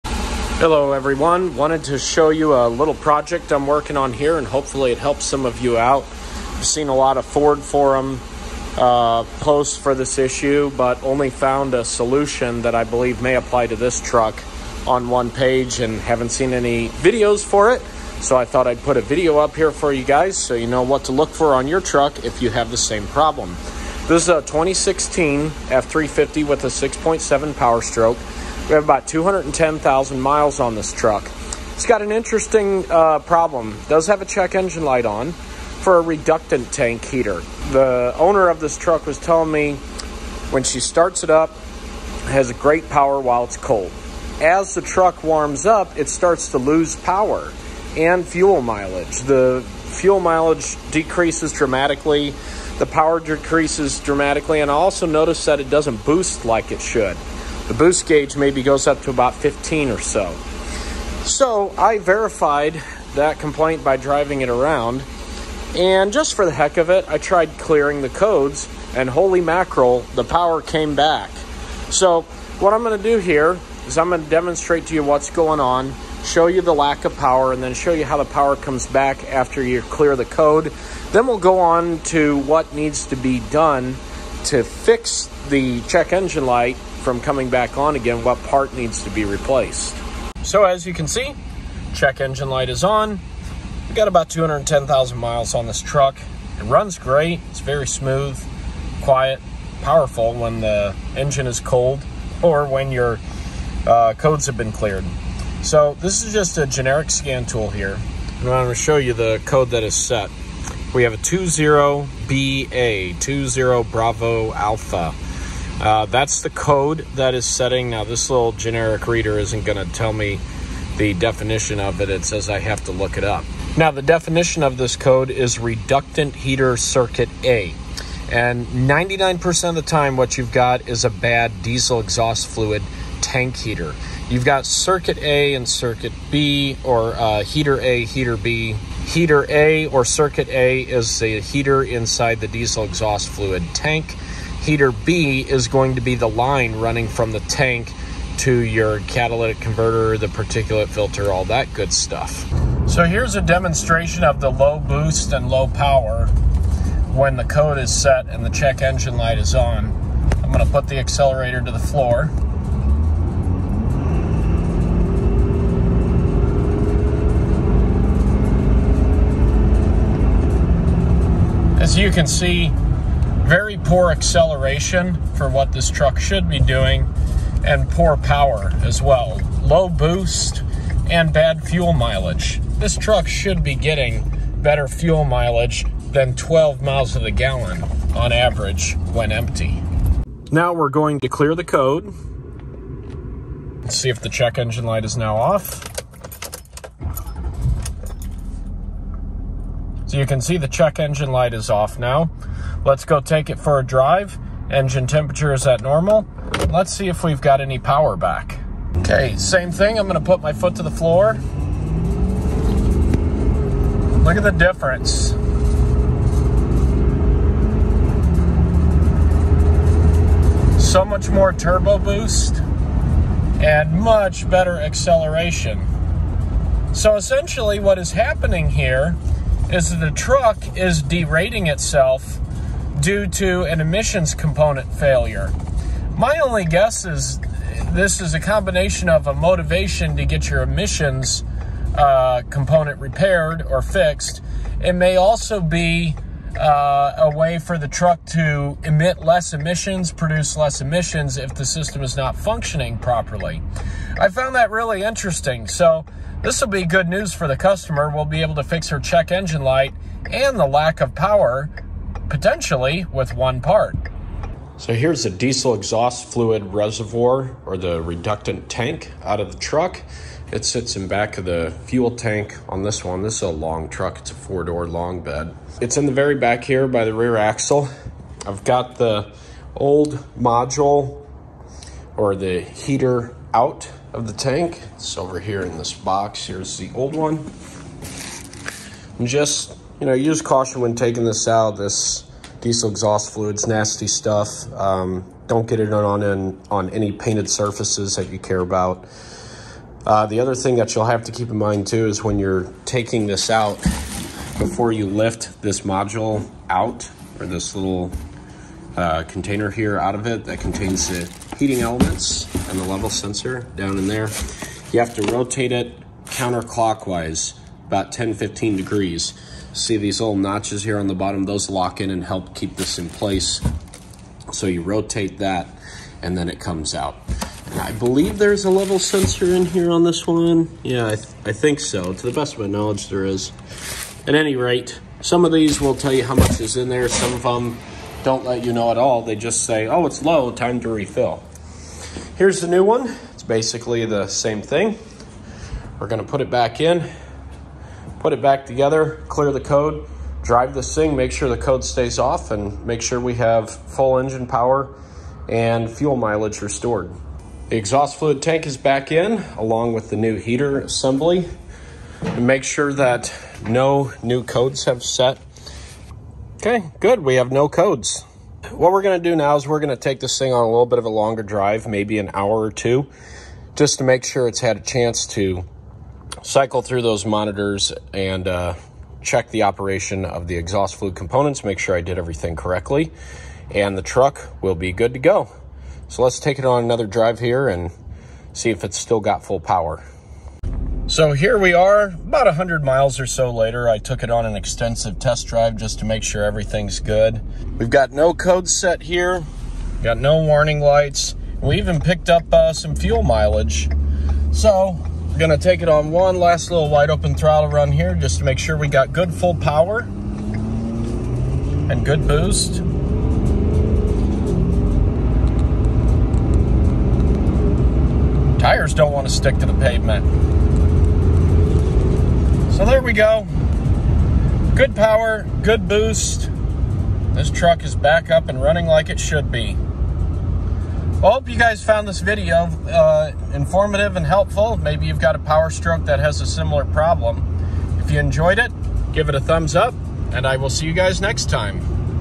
6.7 Powerstroke Low Power - Low Boost - Engine Light FIX
This 2016 F-350 had an issue with low power, low turbo boost when cold and a check engine light, accompanied with poor fuel mileage. The issue turned out to be a faulty diesel exhaust fluid tank heater.